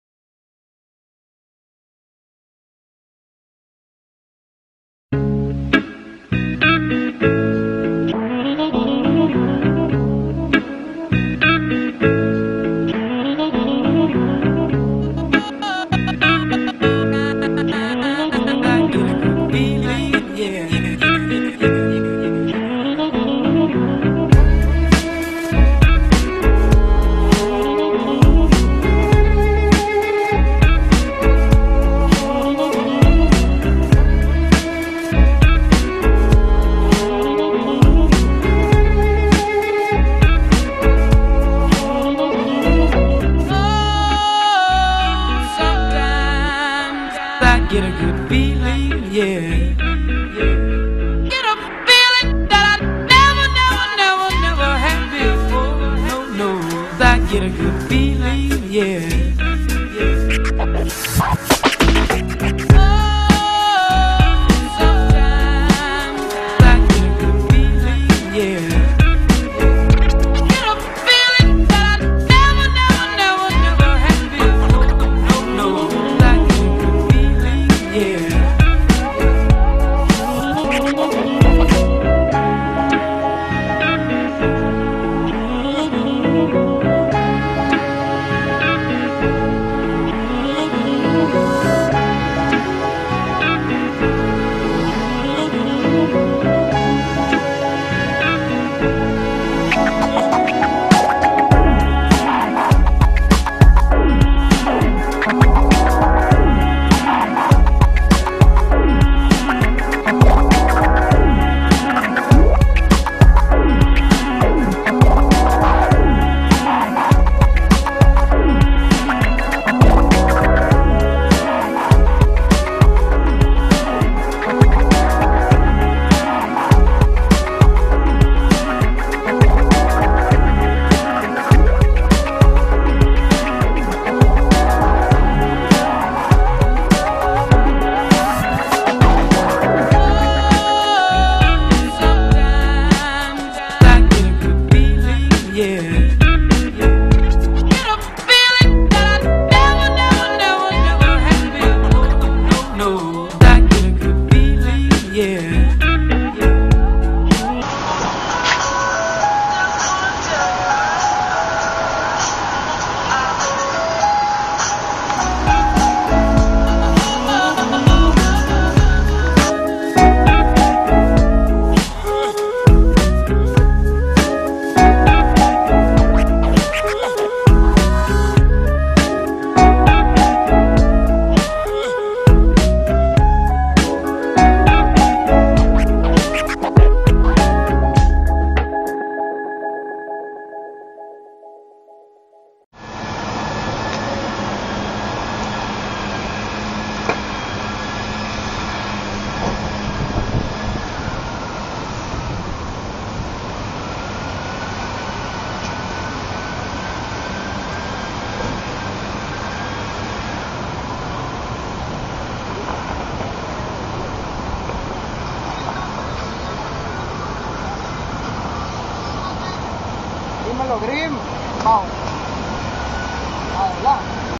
"A day in The Life of NewJack!"
GrimmJackTv presents "A Day in the life" of Bboy Jon NewJack from "RubberClownz" Clan, who shows us his passion for dancing.
Camera: Edwin Rodriguez (Eddie Grimm)